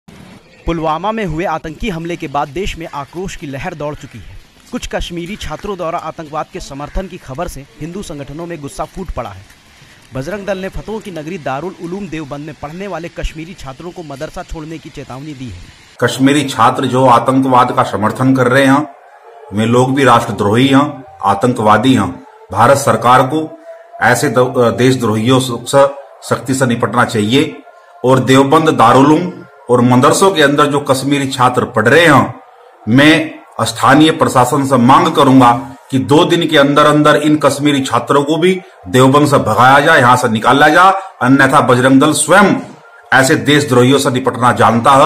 बजंरग दल की चेतावनी, देवबंद से निकाले जाएं कश्मीरी छात्र
सहारनपुर: पुलवामा मे हुऐ आतंकी हमले के बाद देश में आक्रोश की लहर दौड़ चुकी है. कुछ कश्मीरी छात्रों द्वारा आंतकवाद के समर्थन करने की खबर से हिंदू संगठनों में गुस्सा फूट पड़ा है.  बजंरग दल ने फतवों की नगरी दारुल उलूम देवबंद में पढ़ने वाले कश्मीरी छात्रों को मदरसा छोड़ने की चेतावनी दी है.